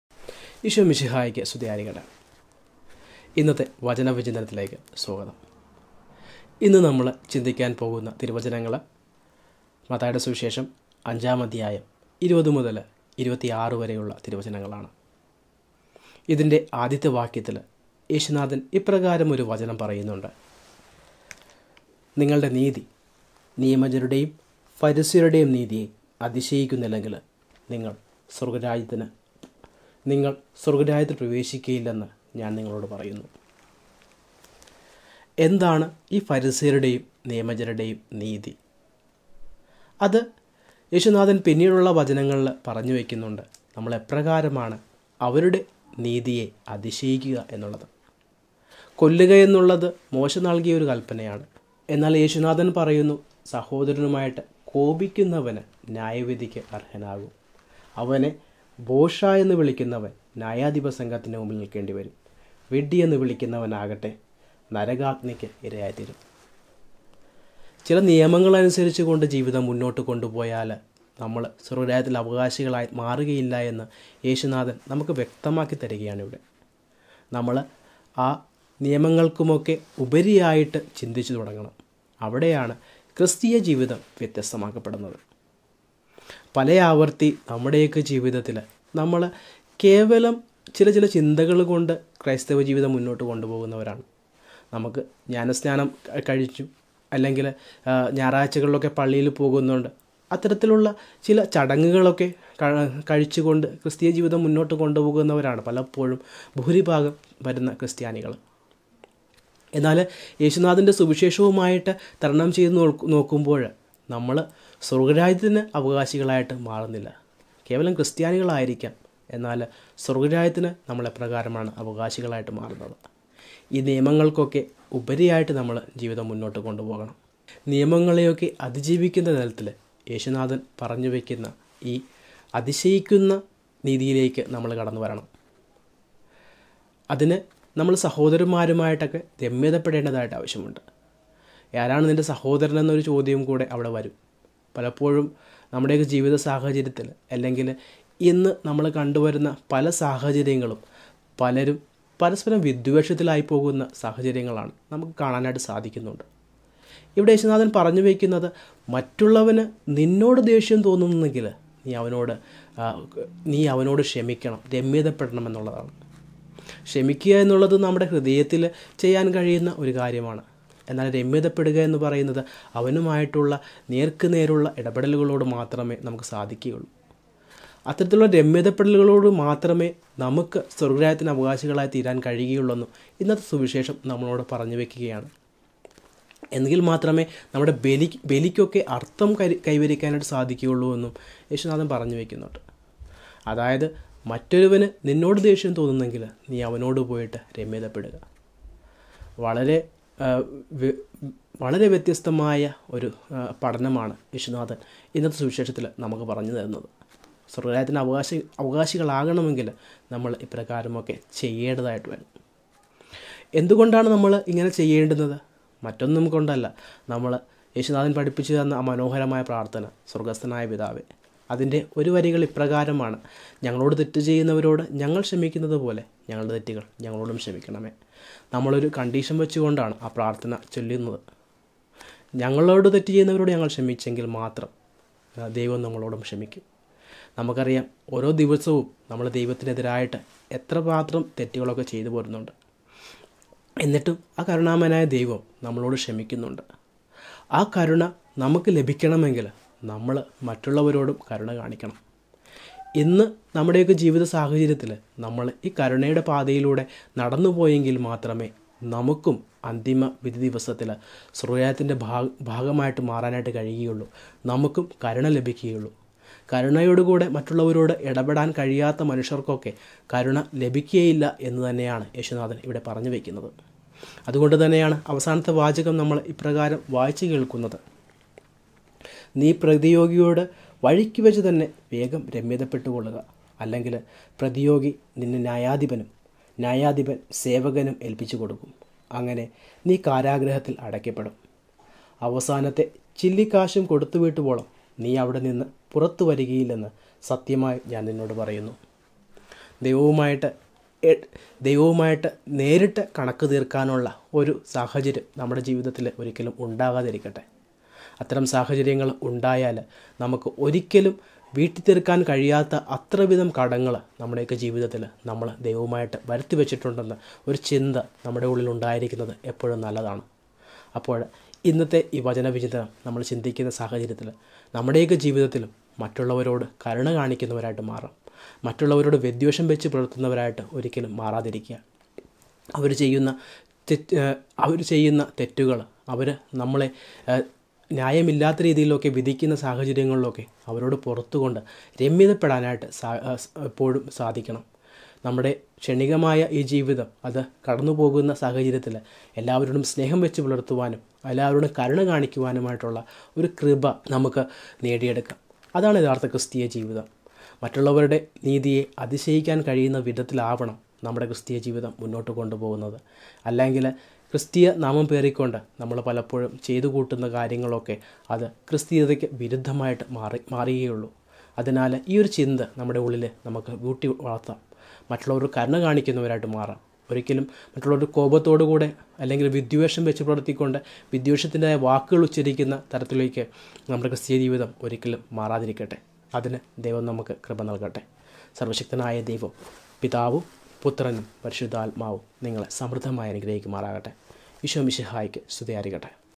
Matthew 5:20-26 Malayalam Reflection | Fr. Stebin John OFM Cap
Daily Gospel Reflection in Malayalam | Malayalam Talk

Each day let us reflect on the Gospel readings of the day to connect with God in our life.